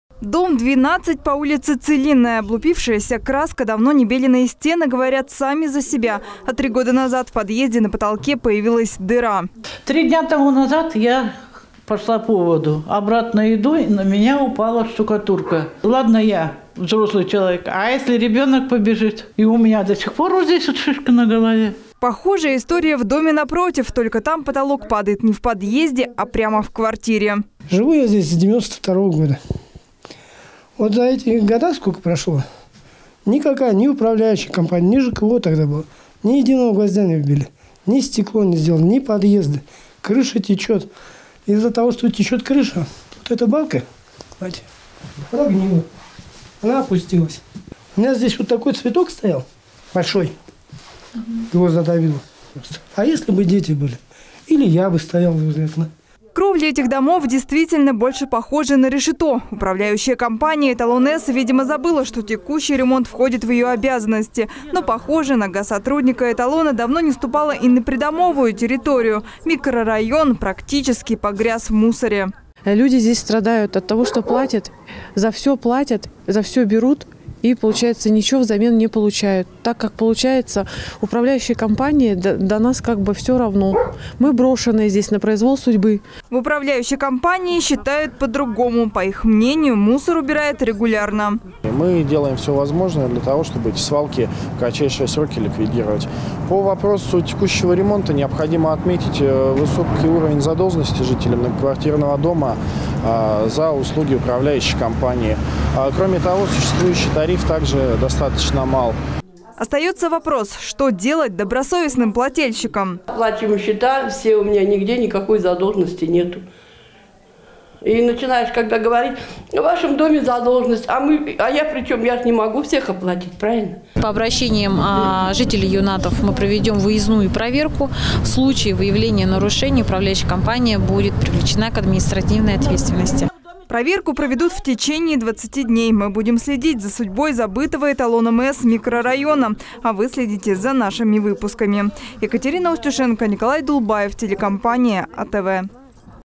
Жители микрорайона Орешково в Улан-Удэ тонут в мусоре
Забытые и брошенные. Так называют себя жители улиц Юннатов и Целинной. Специалисты управляющей компания «Эталон-С» то ли не помнят дорогу, то ли специально обходят проблемные дома стороной. Их давно пора отремонтировать и привести в порядок придомовую территорию. Но для начала неплохо было бы убрать мусор во дворе. Наша съемочная группа встретилась с жильцами и выслушала их претензии.